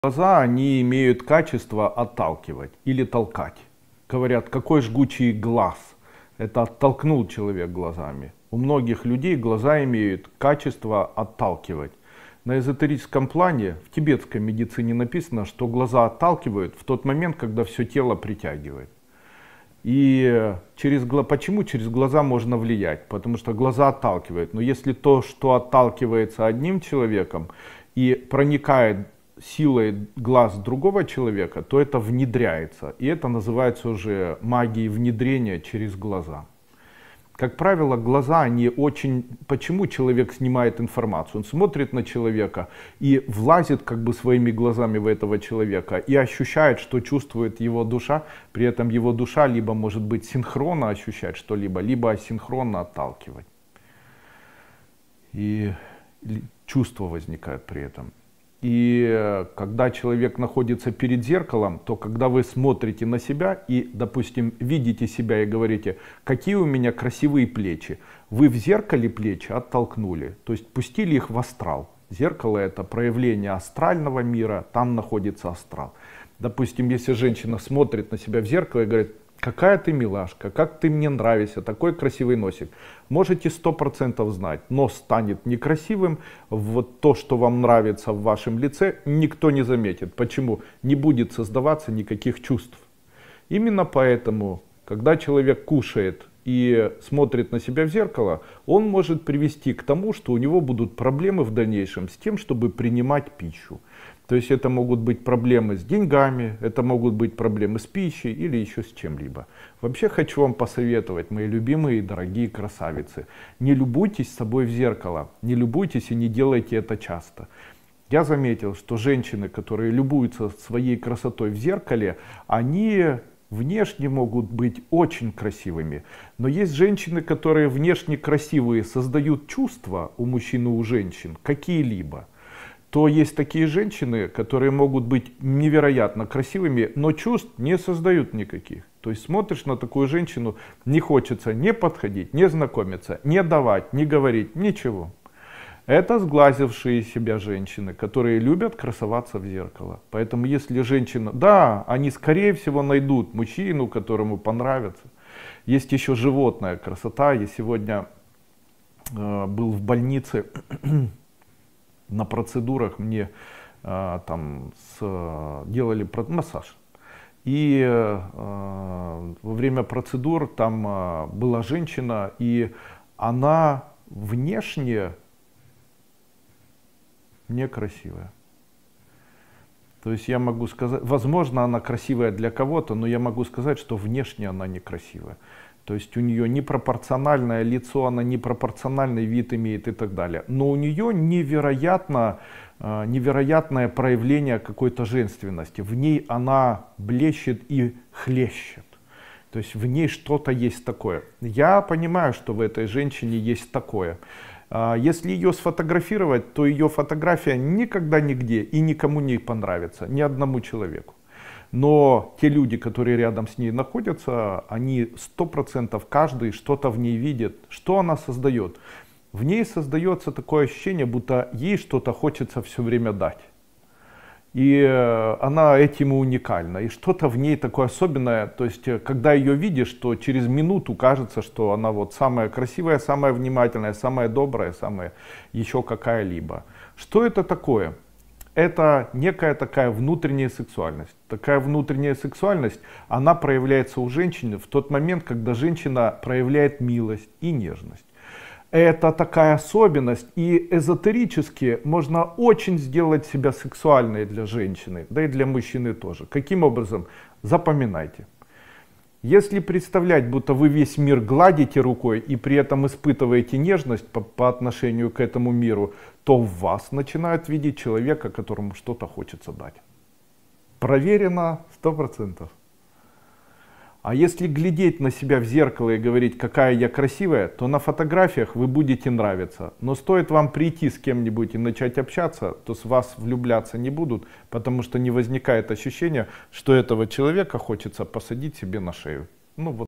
➣Магия внедрения через глаза
Магия Глаз: Как Зеркала и Астральный Мир Влияют на Вас
В этом видео мы исследуем магическое влияние глаз и зеркал на вашу жизнь. Узнайте, как глаза могут служить зеркалом астрального мира и как они могут отталкивать или привлекать энергию. Обсудим, почему через глаза можно влиять на окружающих, и как женственность и сексуальность проявляются в вашем внешнем виде. Также рассмотрим практики, которые помогут вам почувствовать себя более привлекательным и уверенным, как будто вы гладите весь мир.